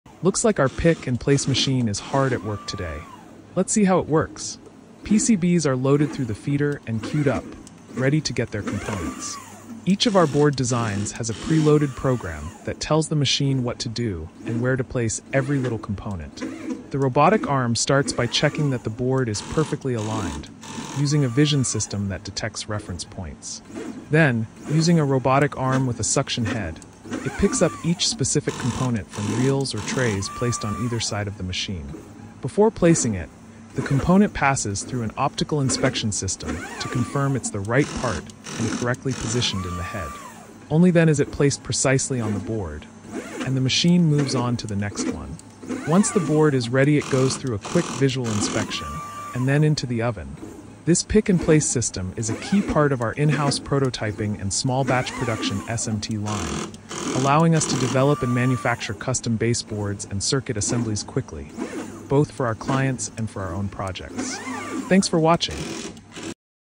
Watch our robotic Pick & Place machine assemble circuit boards 🚀🛠️
Watch components picked from reels and placed on PCBs with pinpoint precision. 🎯

Witness rapid prototyping and small-batch production in action! ✨🔌

Follow our engineering process and subscribe to our newsletter now for updates and exclusive subscribers sneak peeks and future launch early-bird perks.. and be the first to know when we launch!